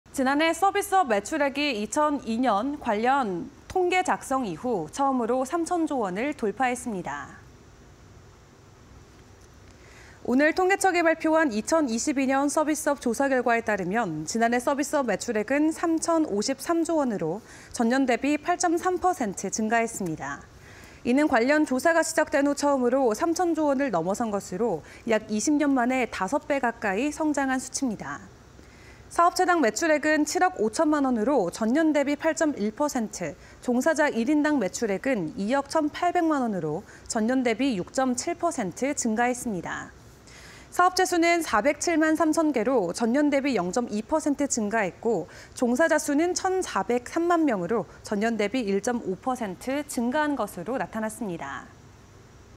코로나 완전히 떨쳐낸 서비스업... 매출액 사상 첫 3천兆 (20231219)
# 코로나 완전히 떨쳐낸 서비스업... 매출액 사상 첫 3천兆